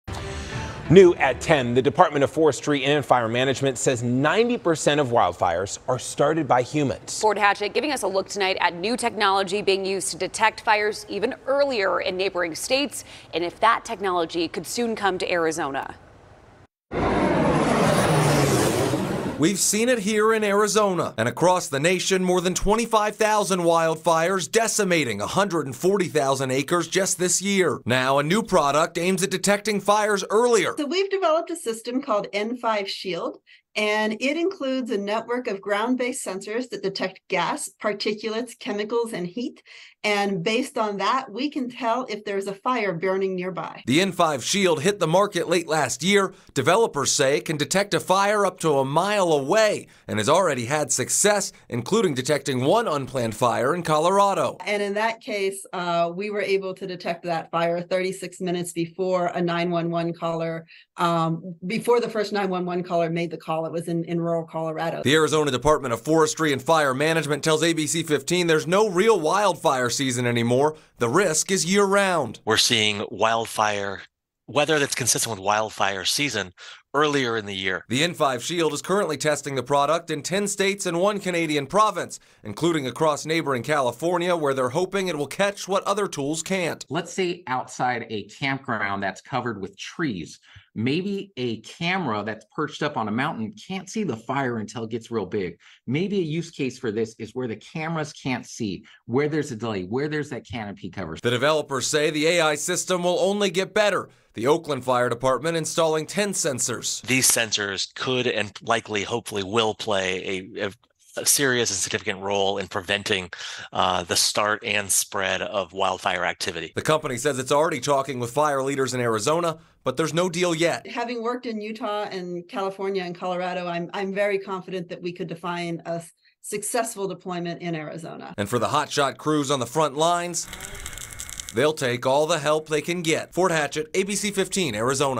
New wildfire detection technology being used in neighboring states
Across the nation, more 25,000 wildfires have decimated 140,000 acres in 2023 alone according to the National Interagency Fire Council.